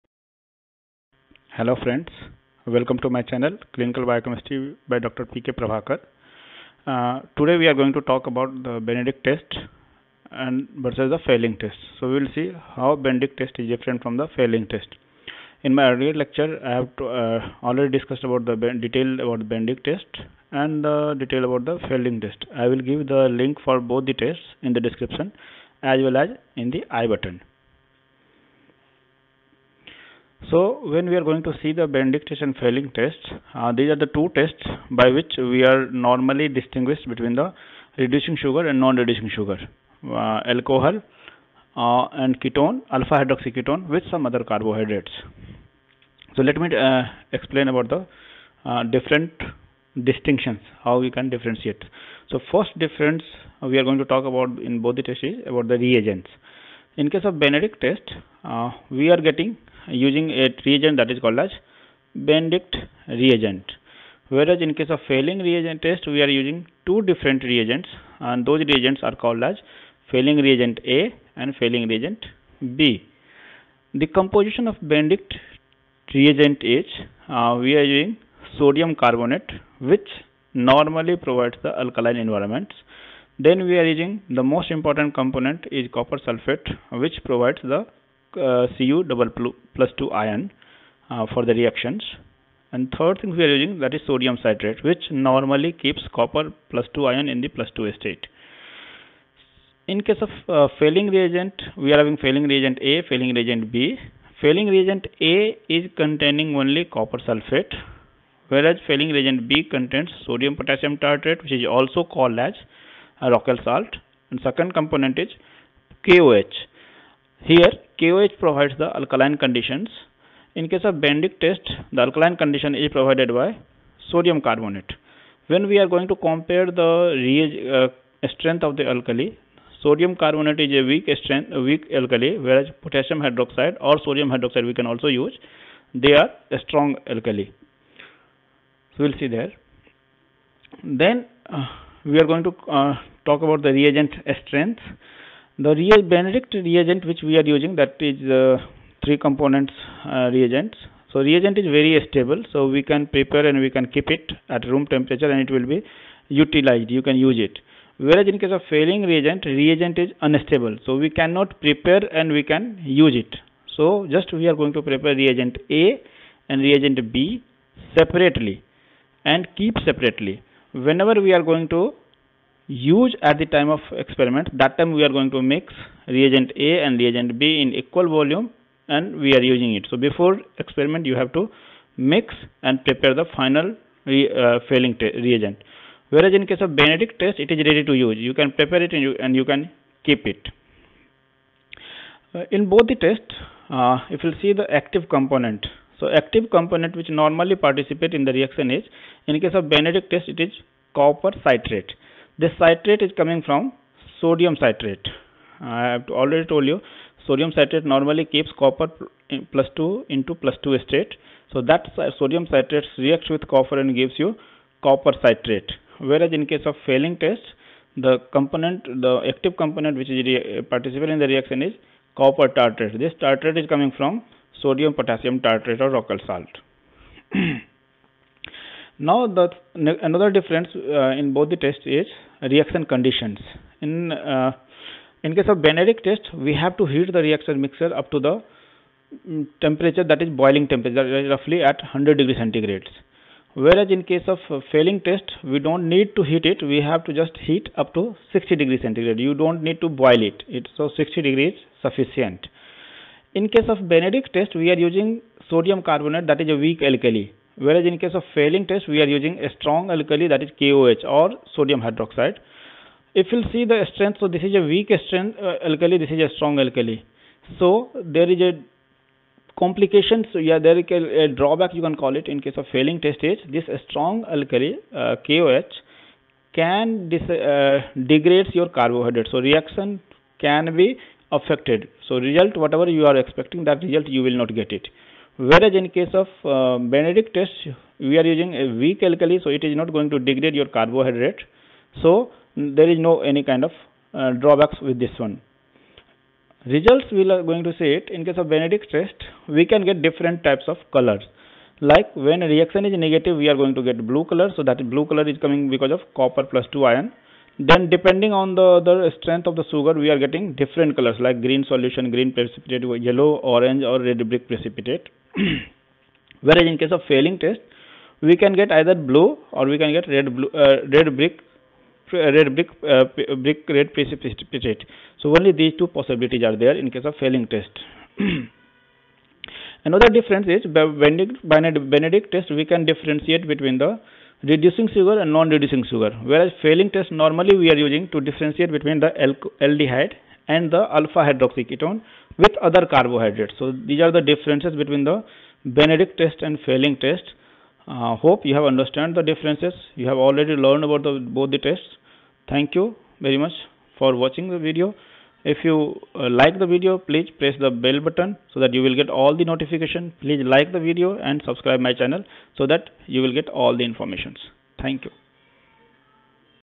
Benedict Test Vs Fehling Test
Hello Friends, Here we are going to discuss the differences between the benedict Test and Fehling Test. We will discuss the differences in the reagents and its composition, mechanism of the reaction, differences in the reaction environment and results.